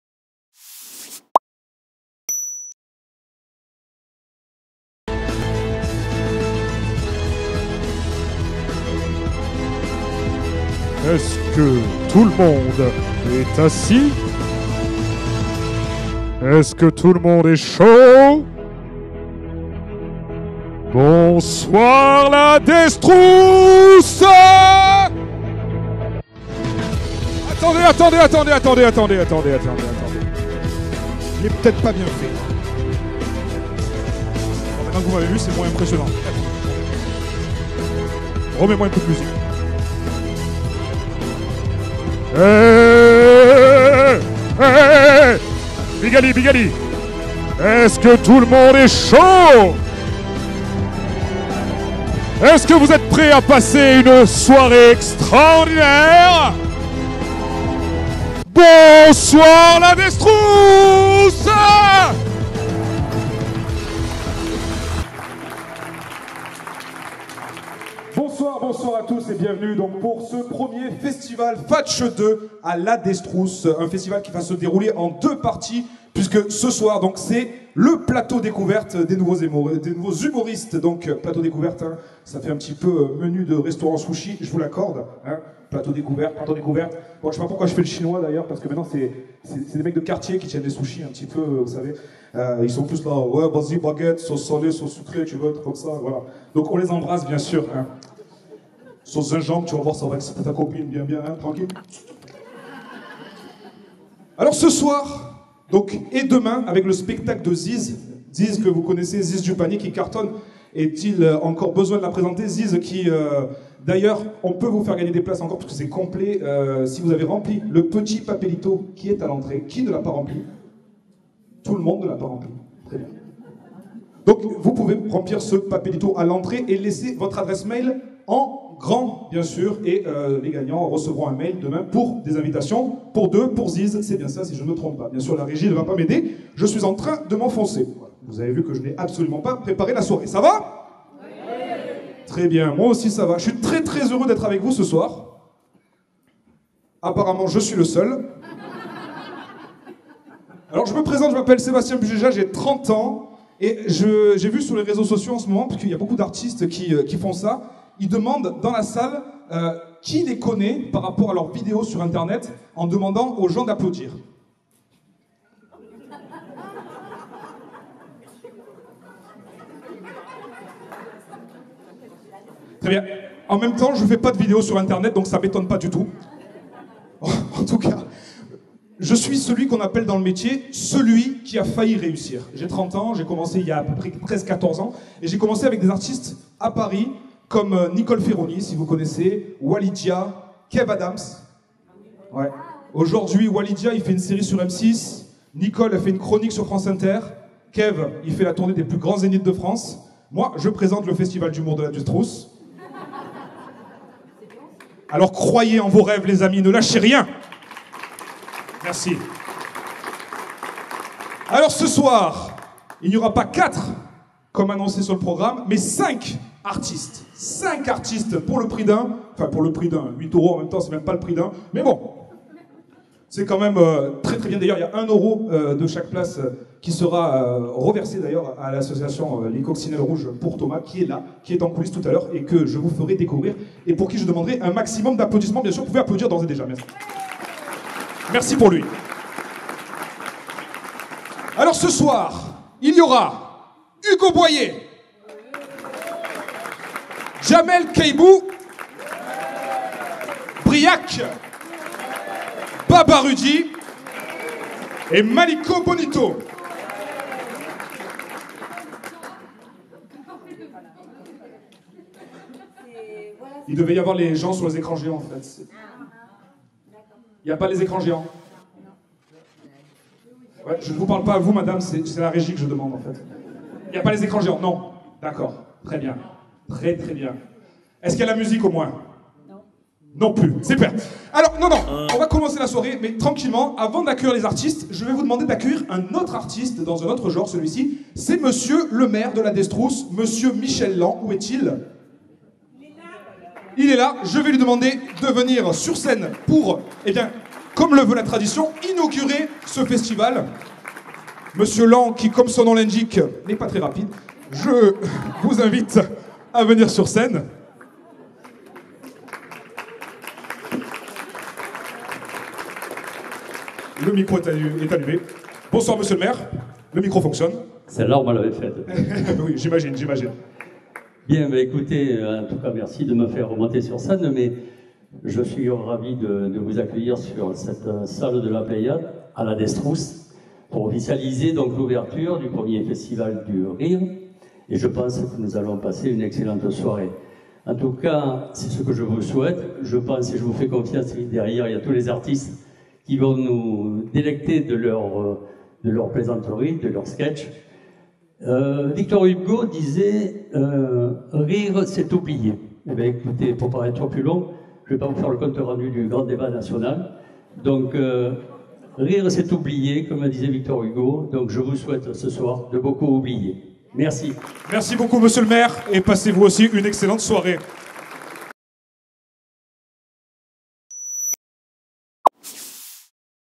Monsieur LAN
Monsieur LAN Maire de La Destrousse inaugure le Festival du Rire. Sébastien BUGEJA, en maître de Cérémonie de la première édition du Festival du Rire de La Destrousse...c'est vraiment n'importe quoi.